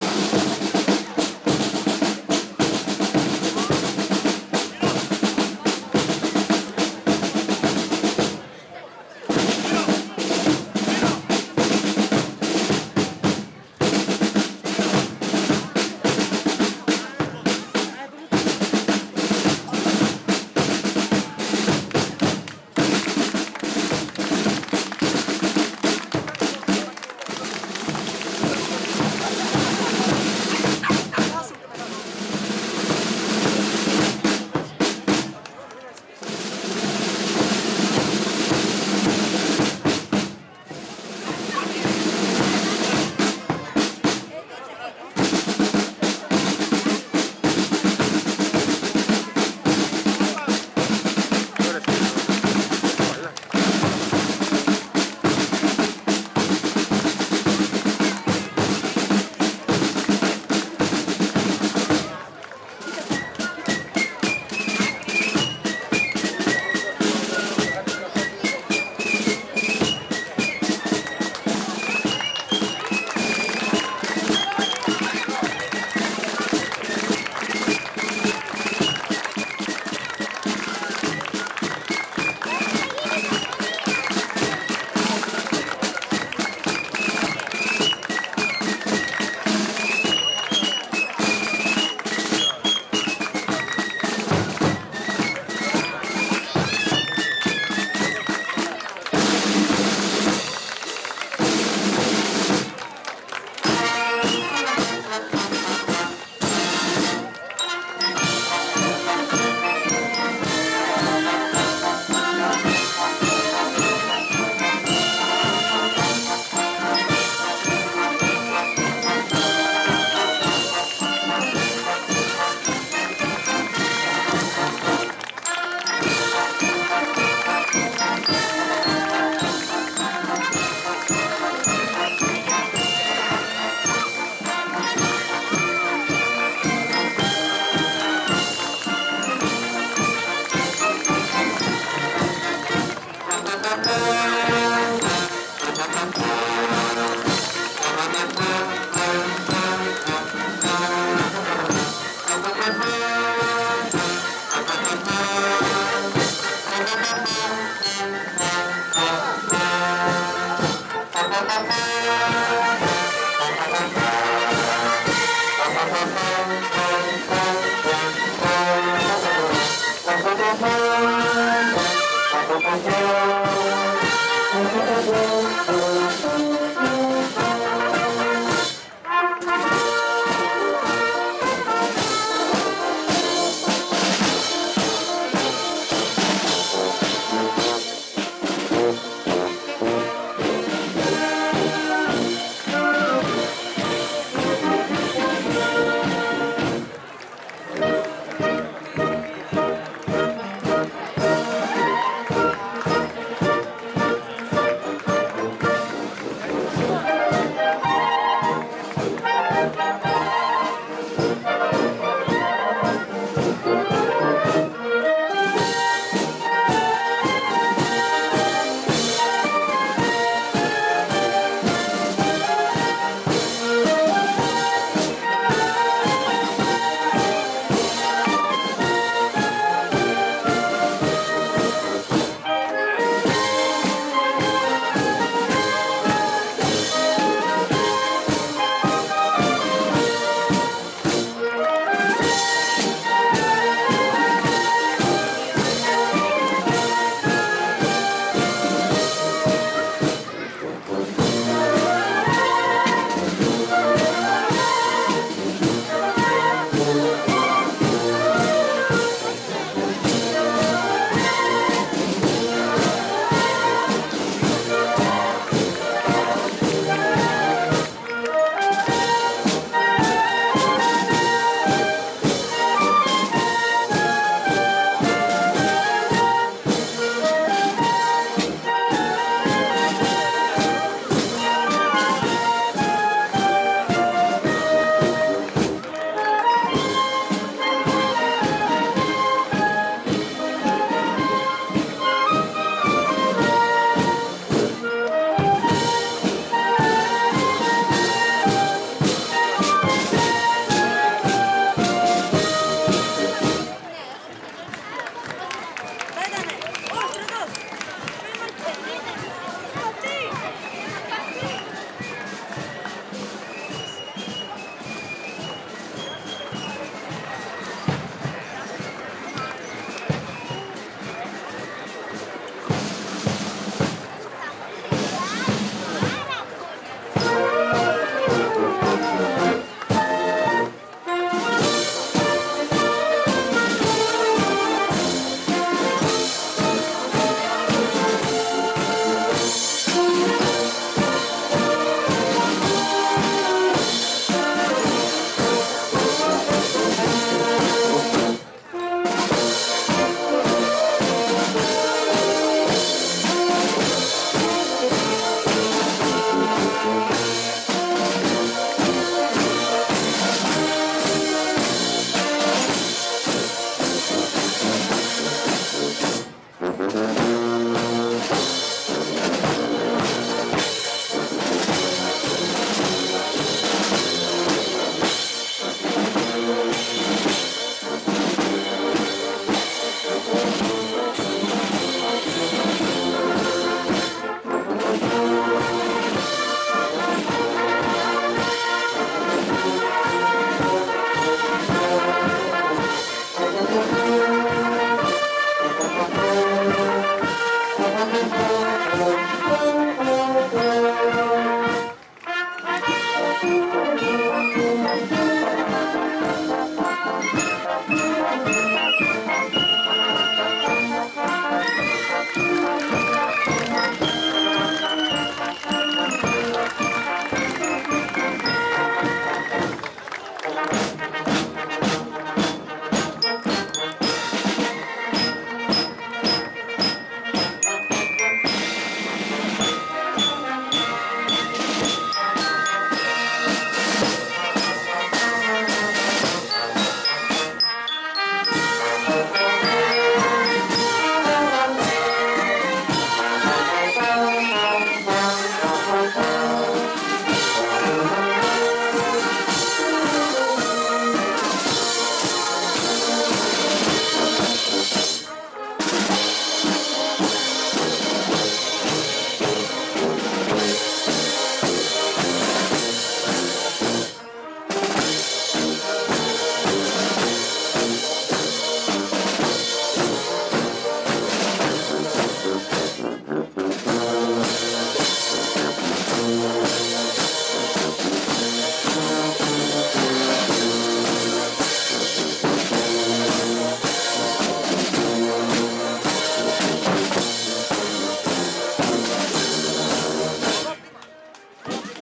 ΦΕΚ 28.10.2021 Παρέλαση
Φιλαρμονική Εταιρεία Κερκύρας
28η Οκτωβρίου 2021 - Παρέλαση
Διεύθυνση: Σπύρος Προσωπάρης
Λήψη: Θεόδωρος Λάκης